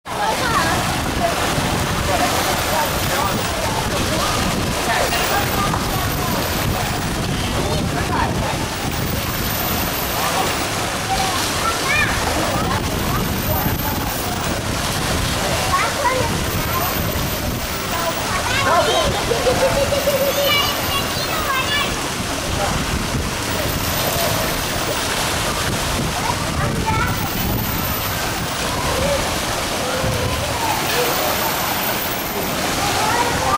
Toboágua
Arthur em seu primeiro toboágua, divertindo-se bastante em nossas férias em Punta Cana.